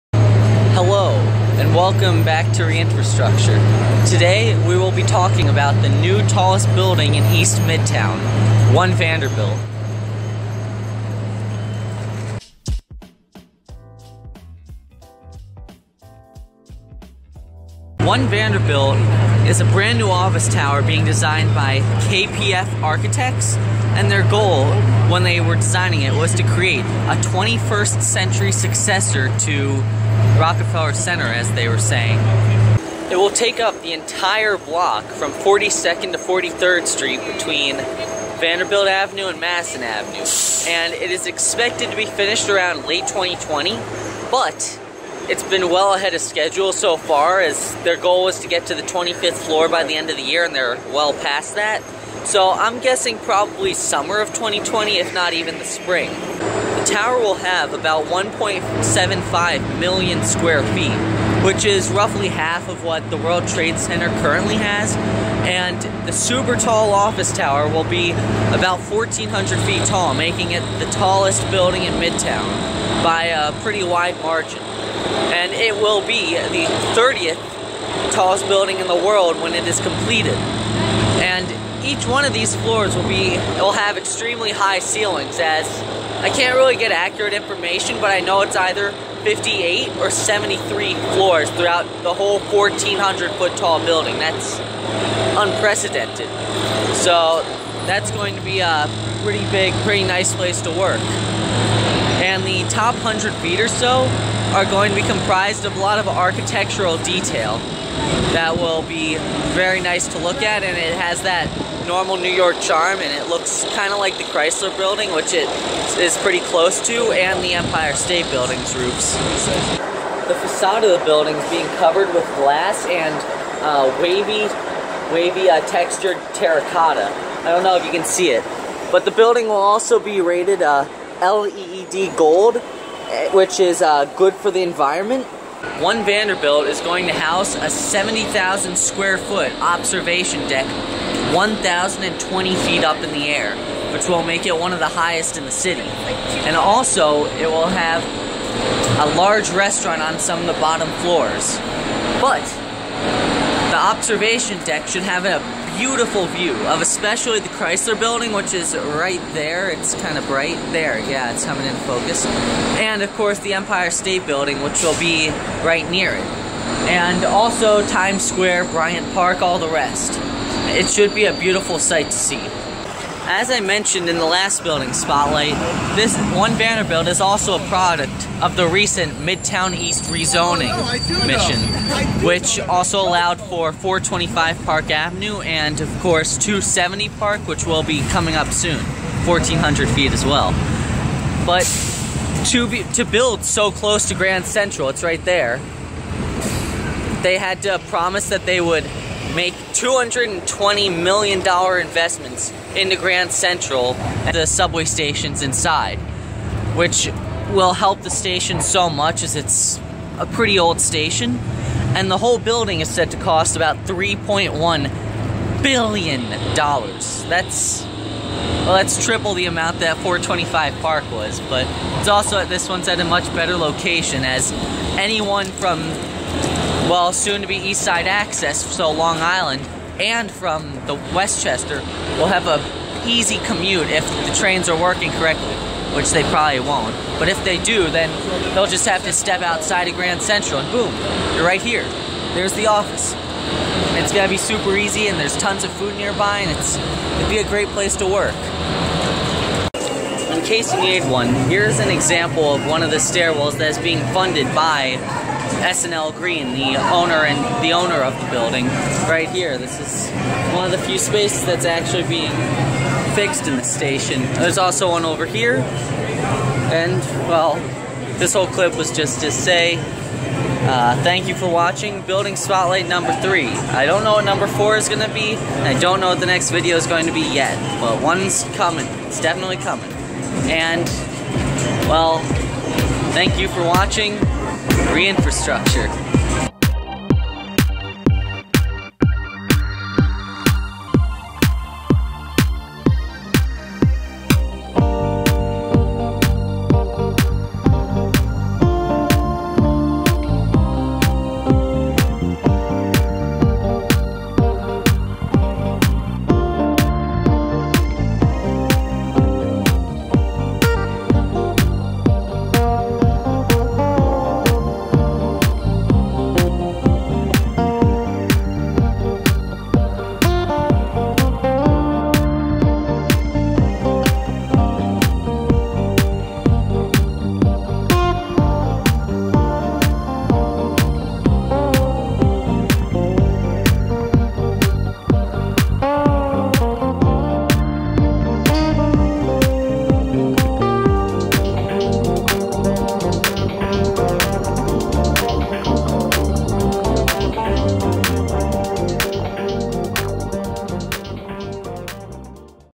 1 Vanderbilt Avenue (Building Spotlight 3)
First, I wanted to fix one mistake. I said the end of the year, and I meant to say at the end of 2018, I had filmed this in December and thought I could get this out by the New Year. They are still well ahead of schedule (in fact they just passed the 800-foot mark) which is nice to hear and I'm keeping my estimated completion date as mid-2020. If you see any other mistakes, then please mention it in the comments.

So with that out of the way, Hello and Welcome to Reinfrastructure! Today we are talking about 1 Vanderbilt Avenue, the newest supertall office building in Midtown. It is one of the first office buildings to be built as a result of the recent Midtown East rezoning which allowed the area to become more competitive with the brand new Hudson Yards project. Adjacent to Grand Central, this 1,400-foot tall monster is going to provide great views for employees and tourists alike. Among the Art Deco classics like the Chrysler Building, Empire State Building, and Rockefeller Center, One Vanderbilt seems to have put a 21st-century spin on this design classic New York design. It will be a welcome addition to the New York skyline.